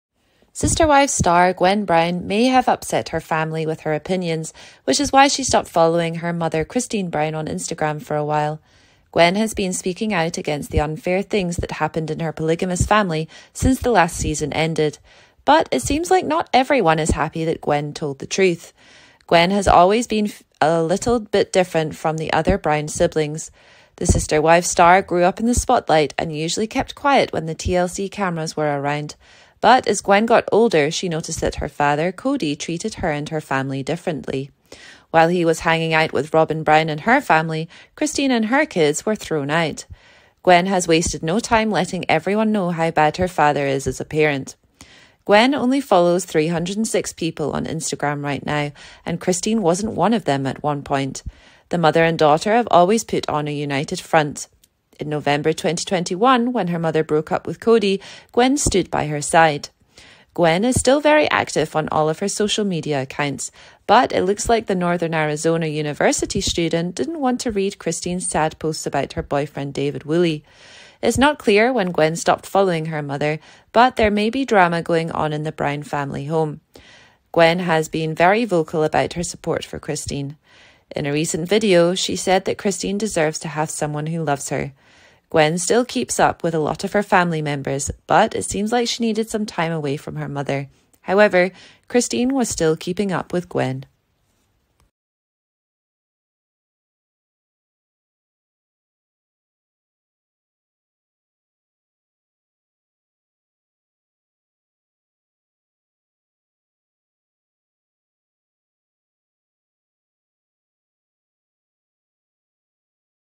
Sister Wives: Gwen Brown Unfollows Christine On IG - New Family Feud?
Sister Wives star Gwen Brown may have upset her family with her opinions, which is why she stopped following her mother Christine Brown on Instagram for a while. Gwen has been speaking out against the unfair things that happened in her polygamous family since the last season ended.